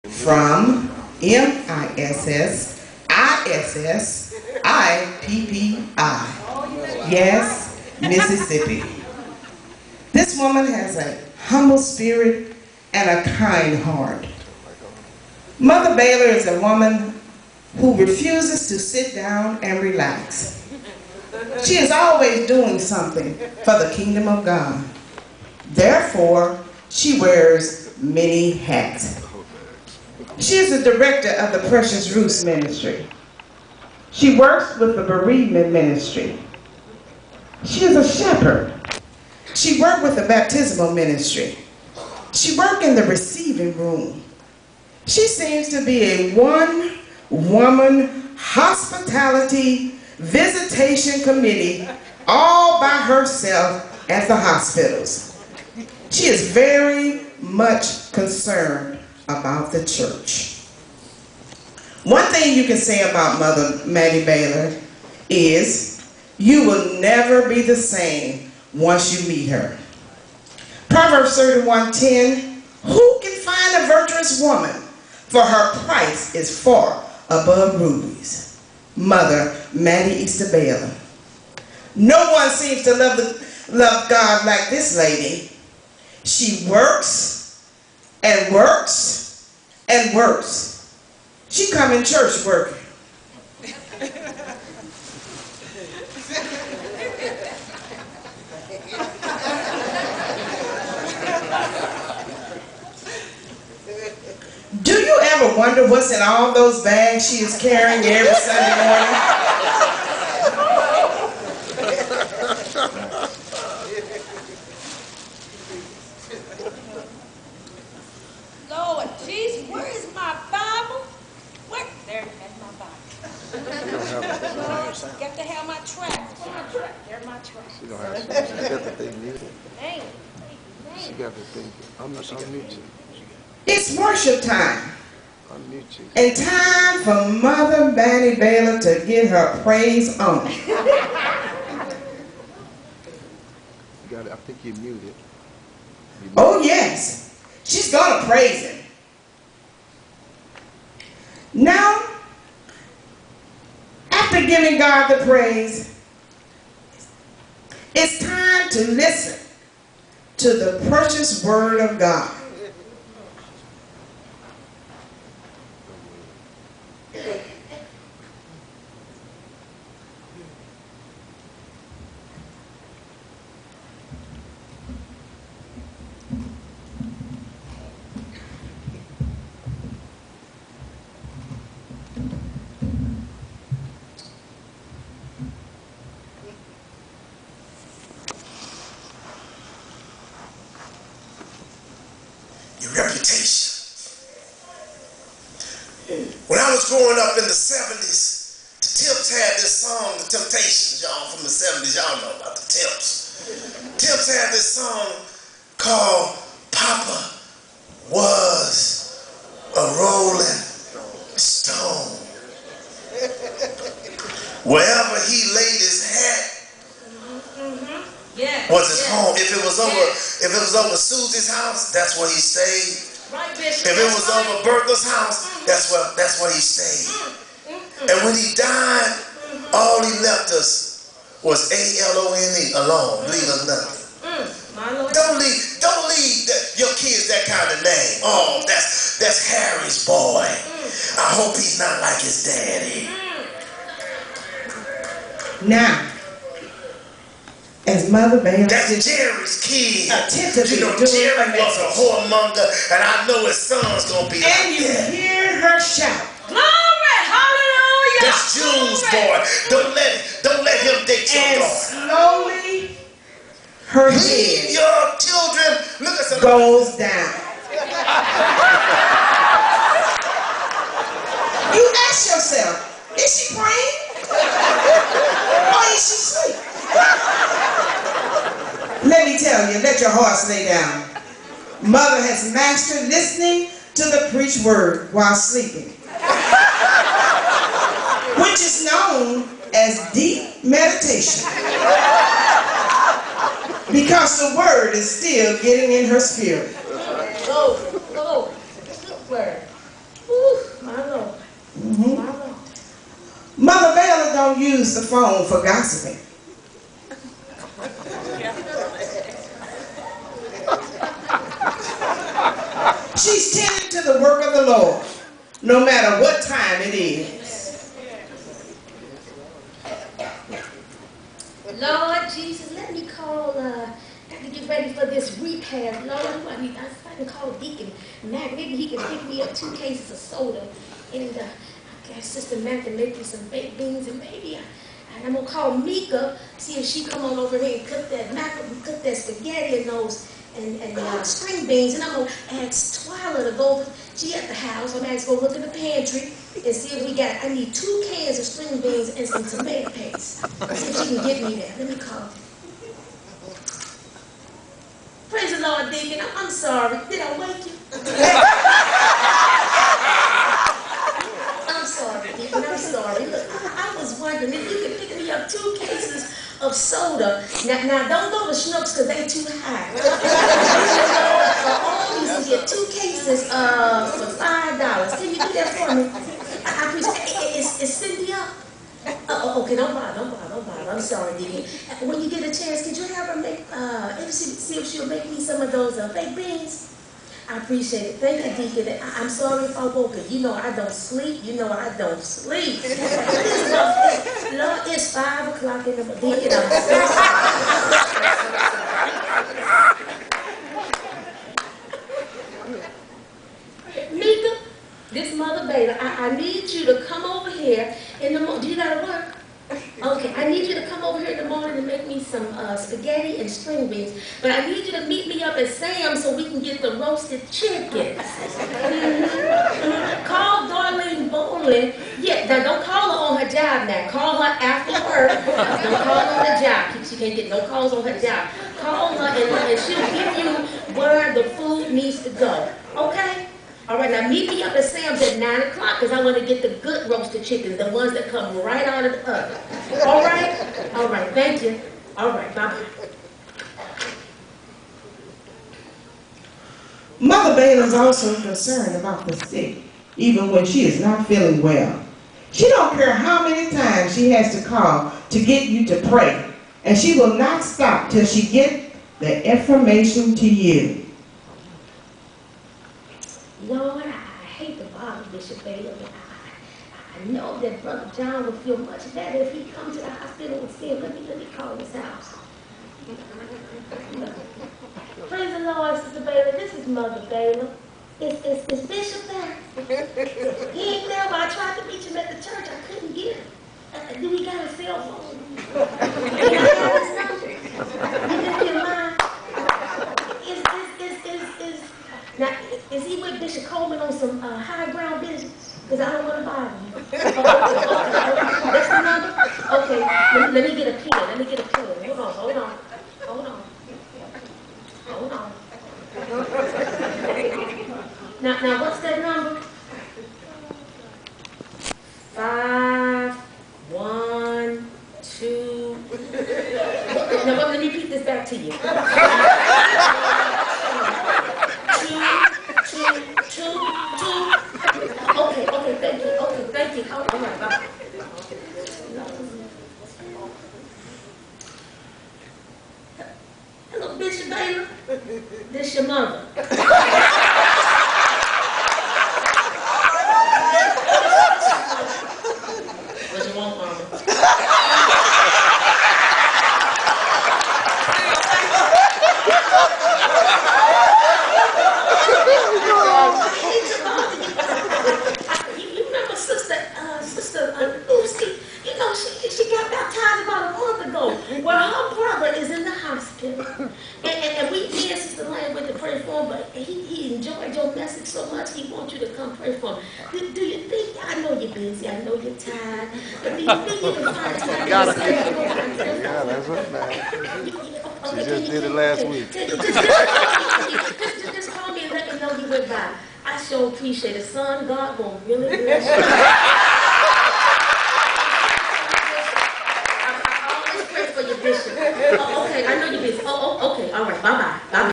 Founder's Day Service 11-22-09 Bishop Larry Baylor Twyla Baylor Sister of Bishop Baylor
Founders Day Celebration  Twyla Baylor portraits Mother Baylor in a Skit in and out of the church.